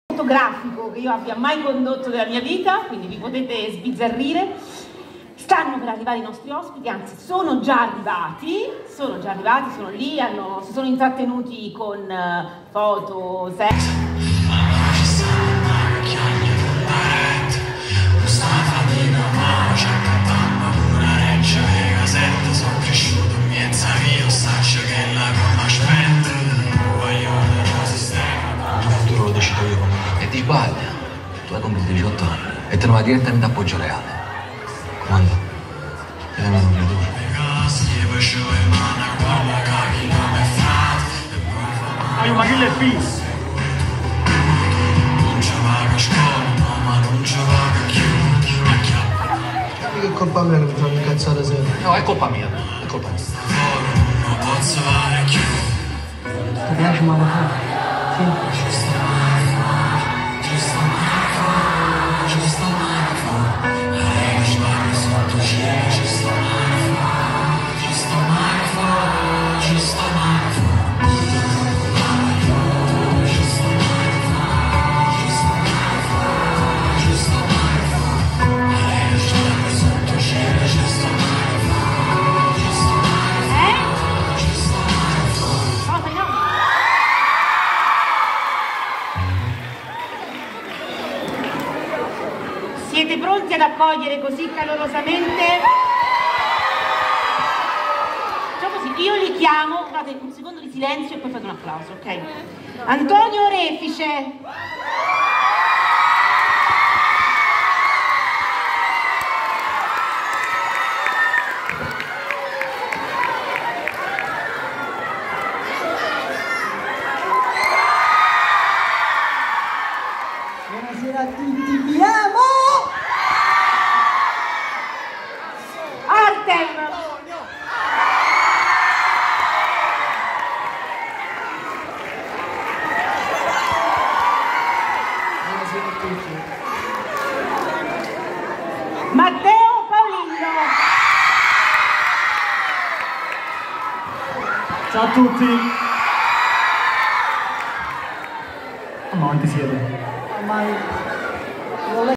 sul palco del social film festival arriva
Antonio Orefice, Artem e @MatteoPaolillo  di  @marefuori6219  incontrano il pubblico di Benevento